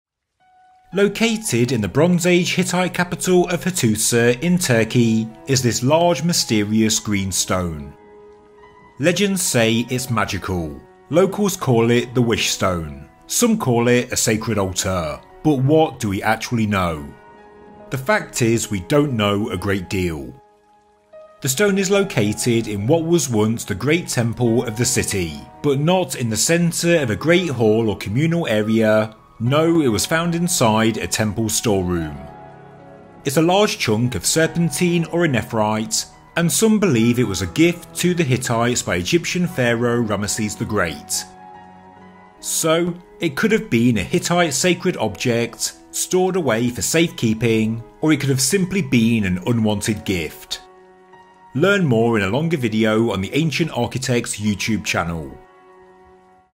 The Mystery of the Green Stone of Hattusa ❇️
Located in the ancient Hittite capital of Hattusa in Turkey, a city build around 1344 BC, is this mysterious large green stone. Legends say its magical, locals call it the wish stone, some call it a sacred altar, but what do we actually know? The fact is, not a lot.

The stone is located in what was once the great temple of the city, but not in the centre of a great hall or communal area, no, it was found inside a temple store room. It’s a large chunk of serpentine or a nephrite and some believe it was a gift to the Hittites by Egyptian pharaoh Ramesses the Great.

So, it could have been a Hittite sacred object, stored away for safe keeping, or it could have simply been an unwanted gift. Learn more on the Ancient Architects YouTube channel.